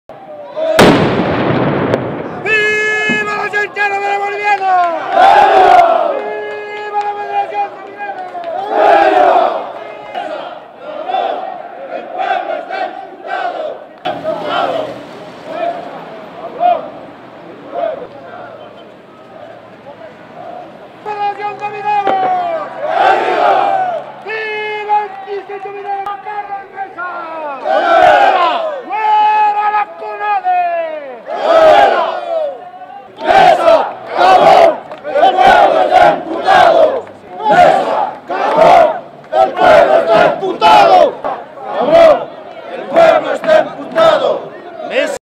Mineros afines al MAS toman por tercer día el centro paceño
Una vez más mineros y otras organizaciones sociales marchan por el centro paceño en respaldo al Presidente Evo Morales.   A pesar de que aseguraron que no usarán dinamita nuevamente hicieron detonar estos explosivos al inicio de su concentración.

Mantienen su estado de emergencia y estarán pendientes sobre los resultados de la auditoría al proceso electoral que realizará la Organización de Estados Americanos.
Entre tanto permanecerán en una vigilia en cercanías de la Plaza Murillo.   Esta vez la FEJUVE de El Alto afín al gobierno se sumó a esta protesta luego de  iniciar su marcha en el multifuncional.